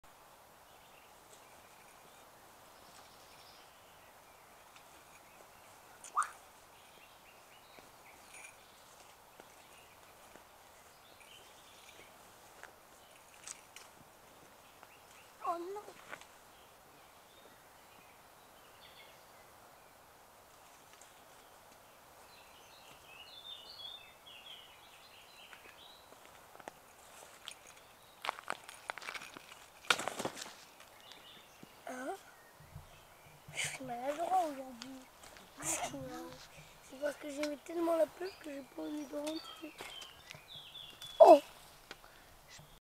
Dada et Nono rentrent de la rivière VDR
Mon fils Noa m'a fait beaucoup rire dans cette vidéo VDR. Je n'écris jamais mdr (mort de rire), mais VDR (Vivant de Rire) car le rire ne tue pas. Au contraire, il vivifie.